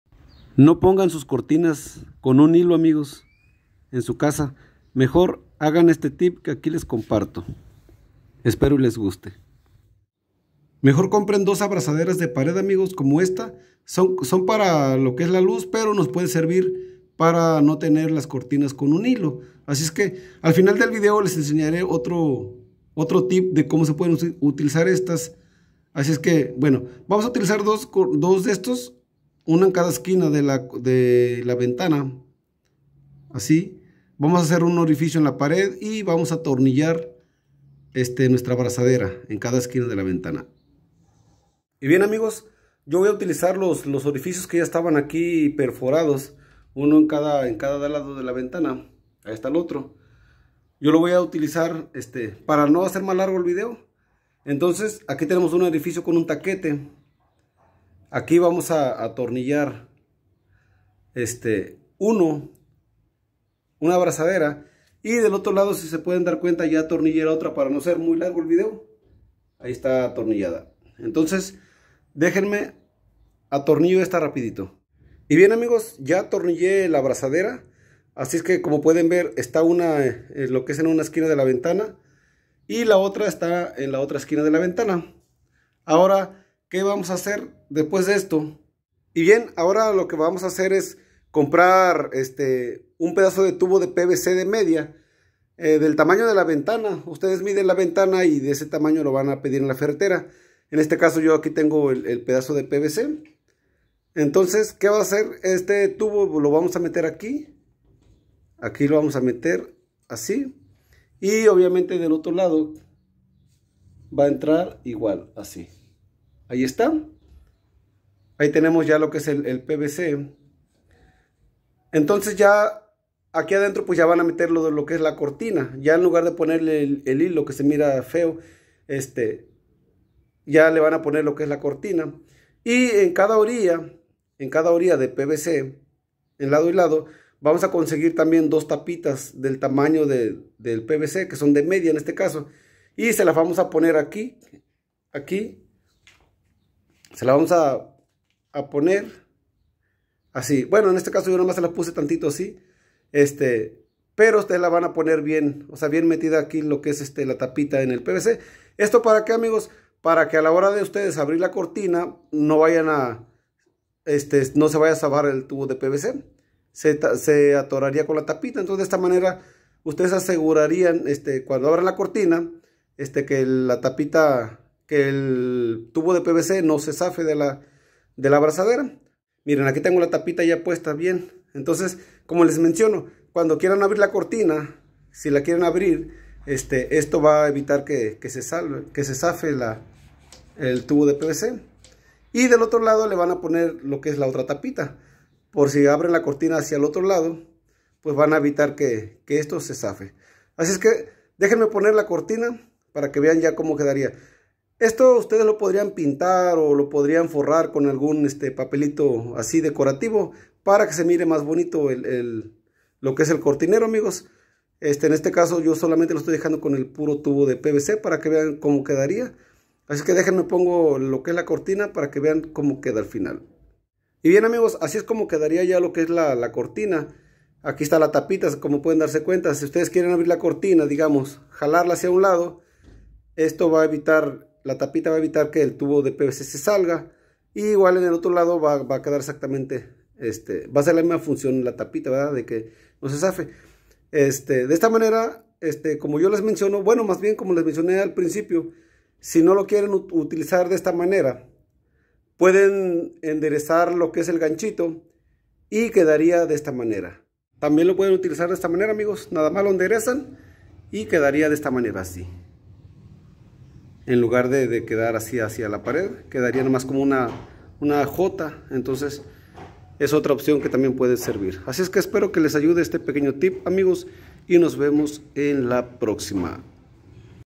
Como Poner Cortineros Para Ventanas fácil y prácticos
En éste video les enseño un tip de como poner Cortineros Para ventanas de una manera muy sencilla y económica, no ponga sus cortinas con hilo,mejor haga éste tip